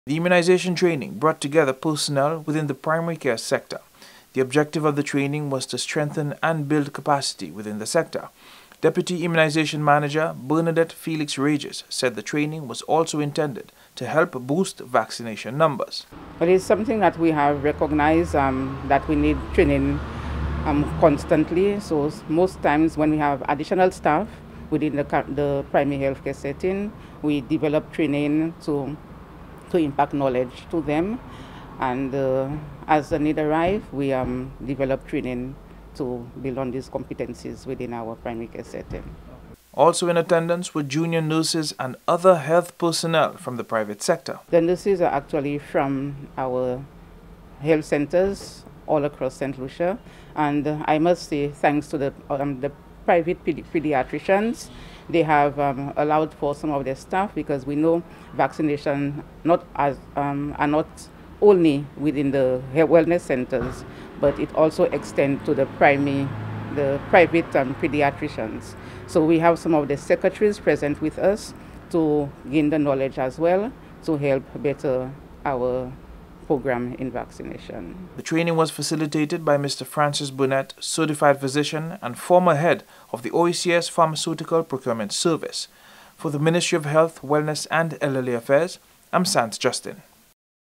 Nurses Immunization Training
Officials within the private and public sector recently received training in the area of immunization. The two-day training which was held at the Coco Palm Resort recently, served as a refresher for some participants, whilst others were first time recipients of the training.